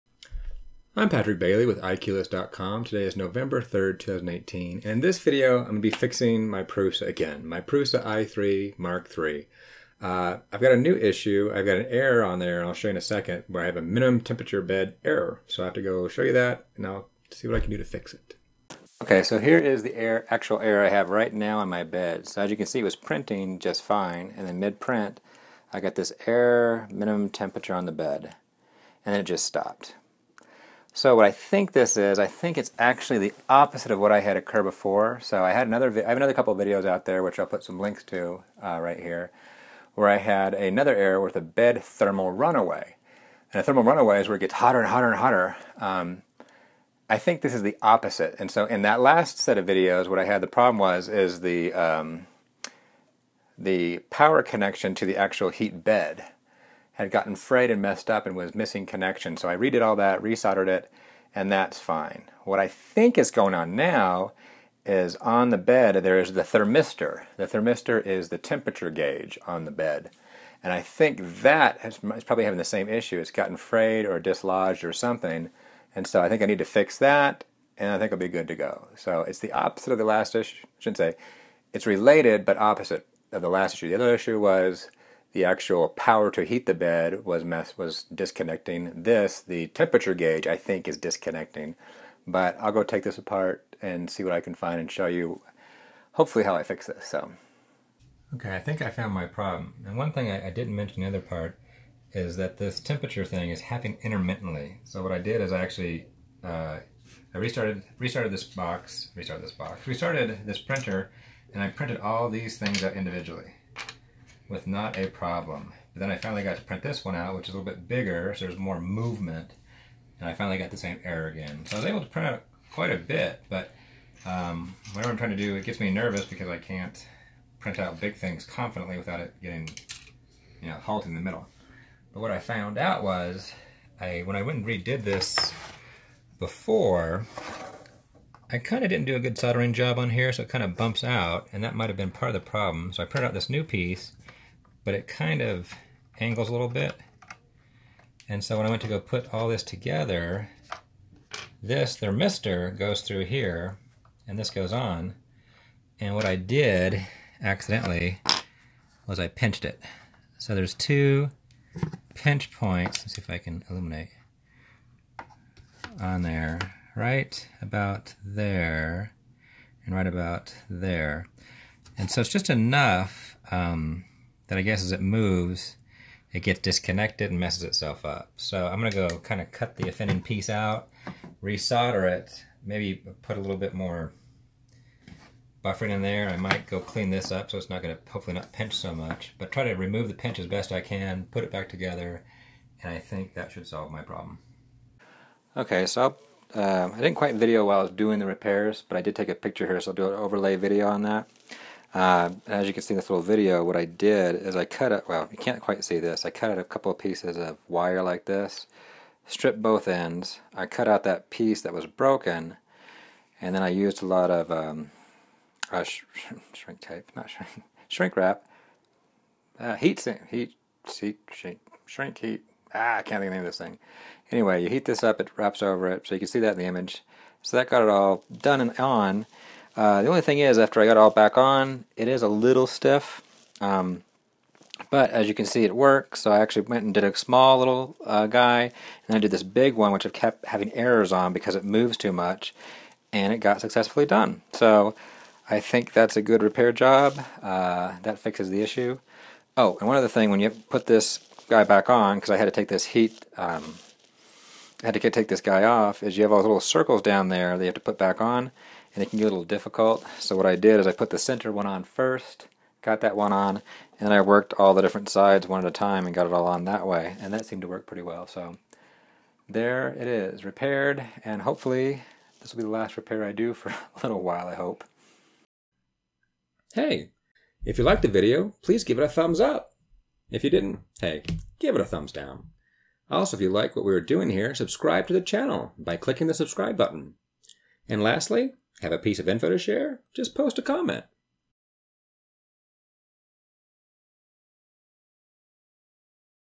Prusa Printer: Issue Err MINTEMP BED
Another Issue with my Prusa Printer, but I got it fixed :)